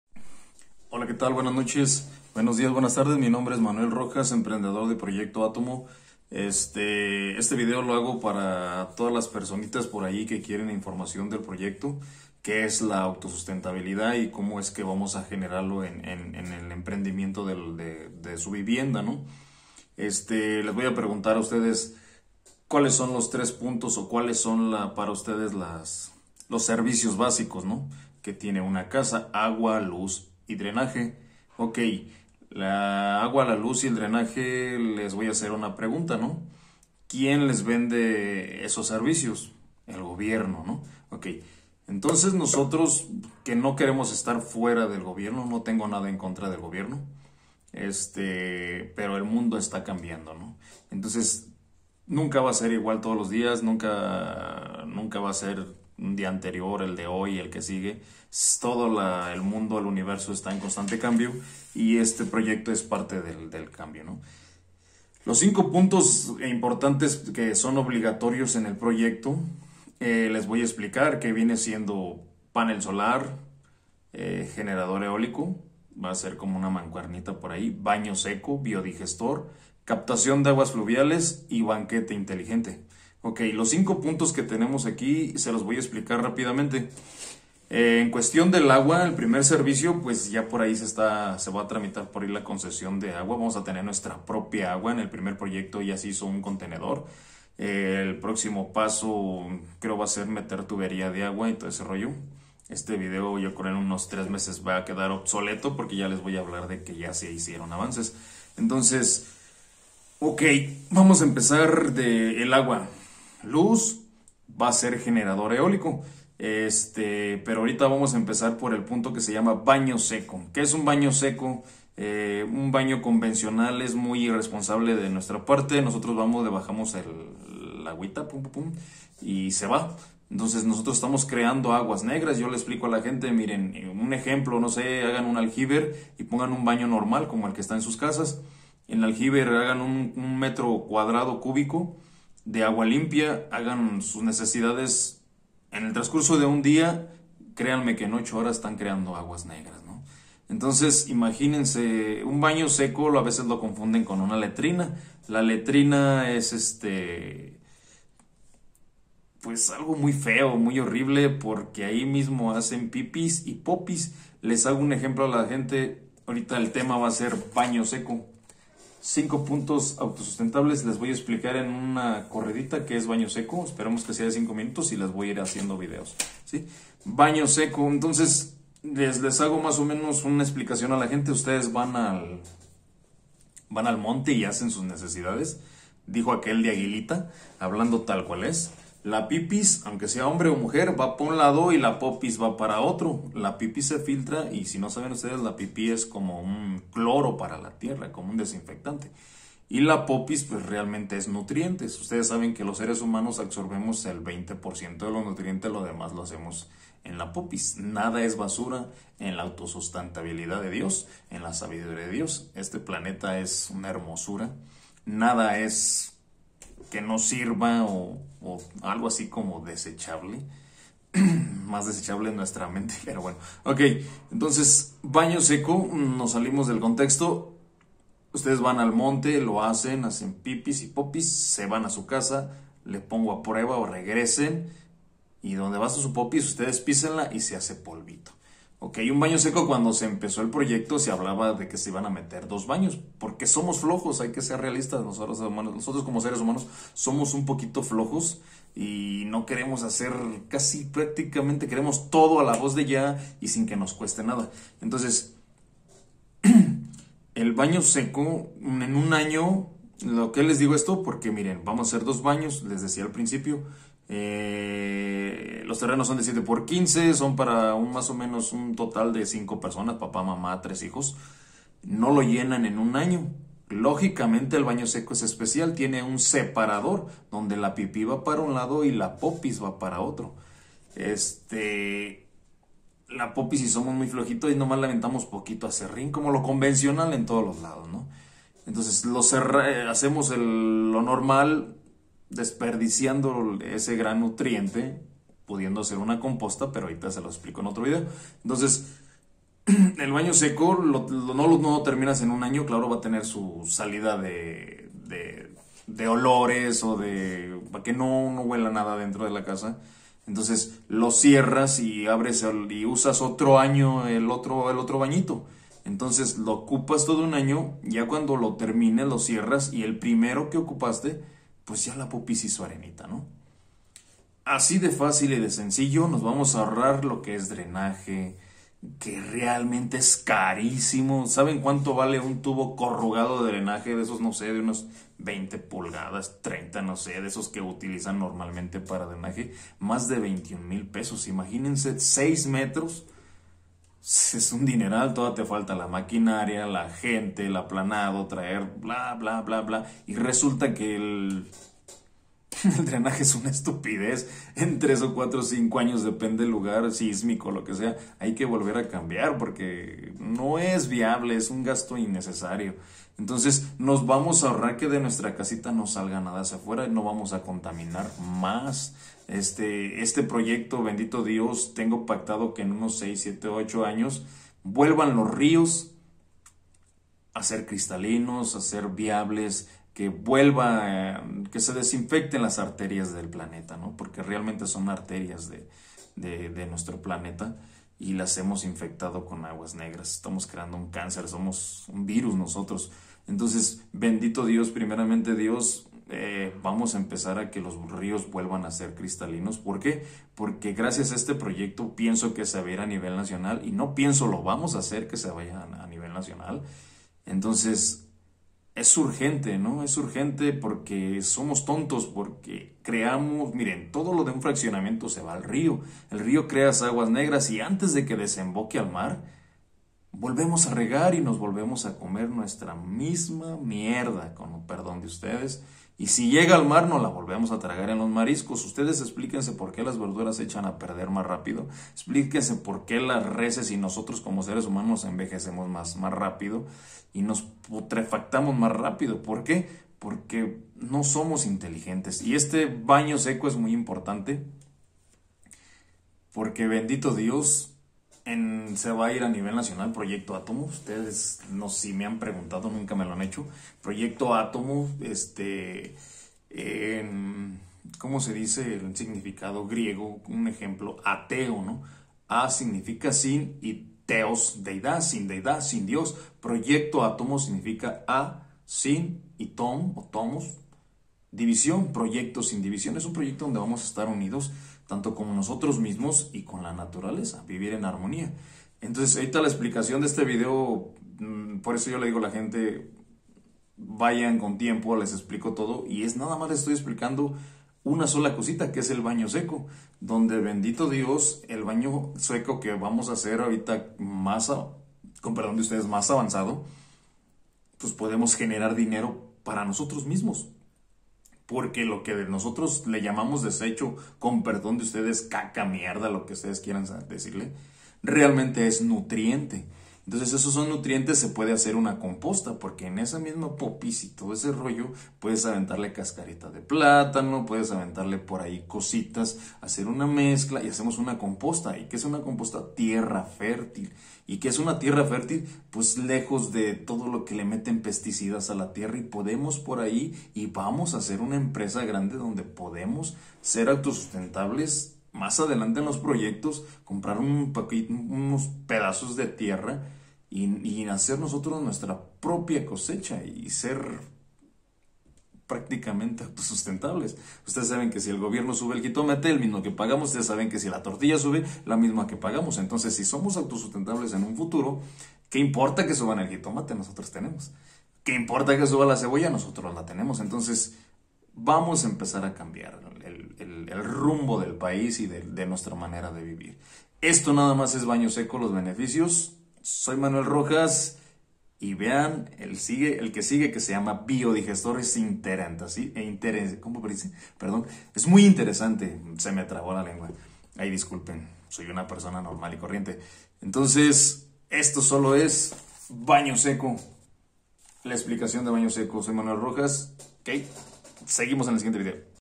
BAÑO SECO
UNA PEQUEÑA EXPLICACION DE LO QUE ES UN BAÑO SECO.
ES UN BAÑO CON SEPARADOR DE PIPIS Y POPIS...
HAY BAÑO DE SUPERLUJO.
VEAN VIDEOS BUENOS POR QUE HAY DESDE LOS SIMPLE A LO BUENO.